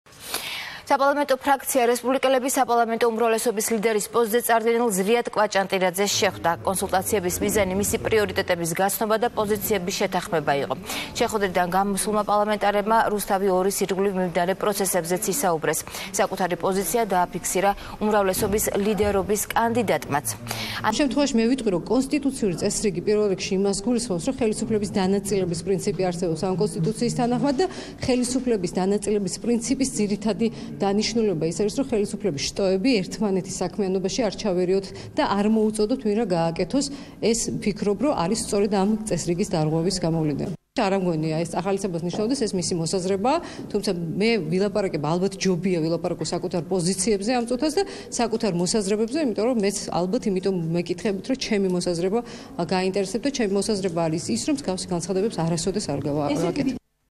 ფრაქცია „ქართული ოცნება-რესპუბლიკელები“ ზვიად კვაჭანტირაძეს შეხვდა
საპარლამენტო ფრაქცია „ქართული ოცნება-რესპუბლიკელები“ ზვიად კვაჭანტირაძეს შეხვდა.

შეხვედრიდან გამოსულმა თამარ კორძაიამ რუსთავი 2_ის ირგვლივ მიმდინარე პროცესებზე ისაუბრა.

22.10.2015